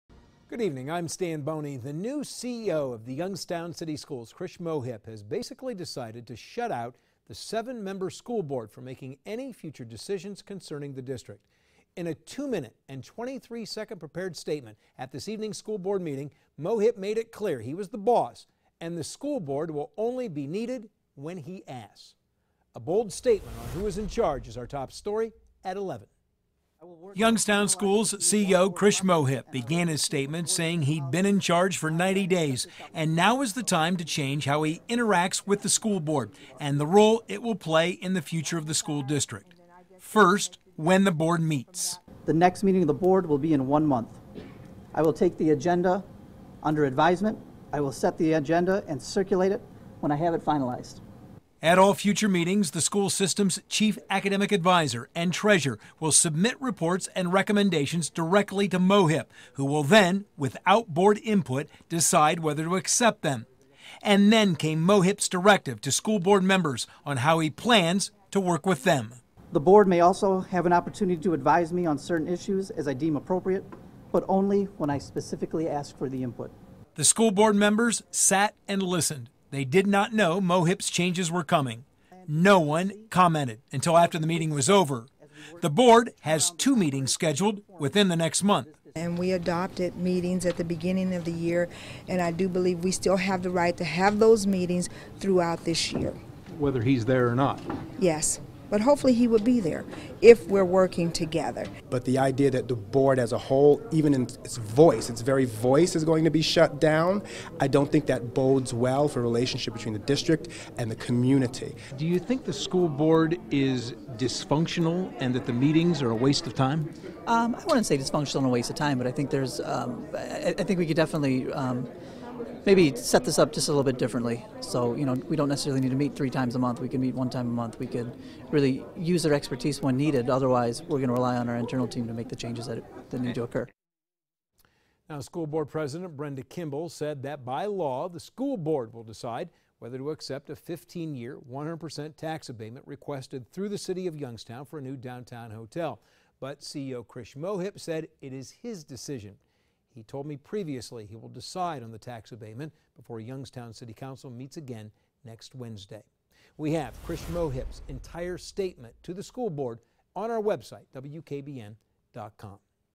Youngstown CEO decides to limit school board’s power
Krish Mohip says the Youngstown School Board needs to be set up differently.